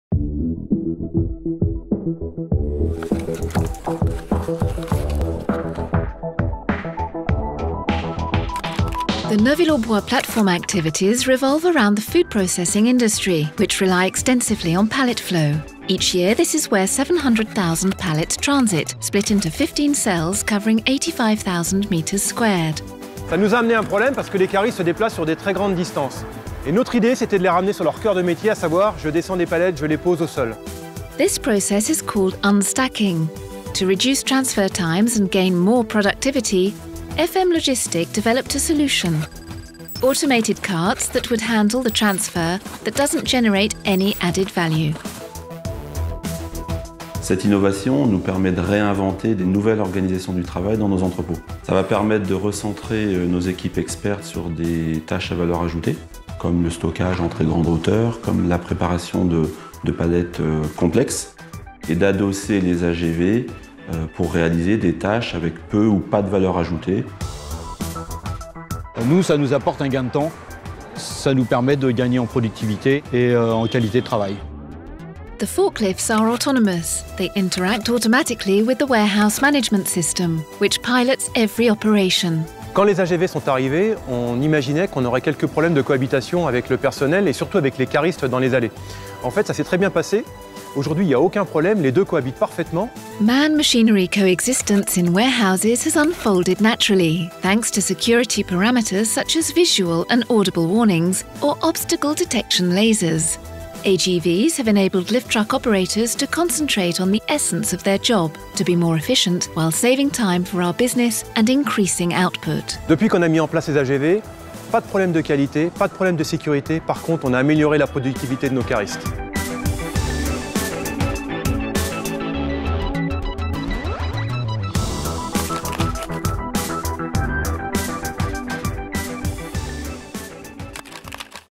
FM AGV (Automated Guided Vehicles) [EN]
This automated solution represents a gain in productivity and freed up time for employees, allowing them to devote themselves to value-added tasks.